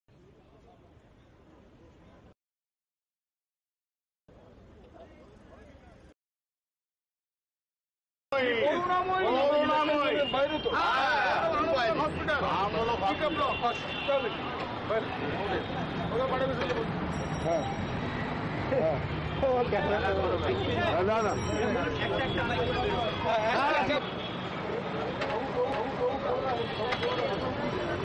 Dilip Ghosh, saltlake tmc leader has been identified as one of the bike-gang members.
Dilip Ghosh, saltlake tmc leader has been identified as one of the bike-gang members. The number plate on the bike shows it belongs to him, say locals.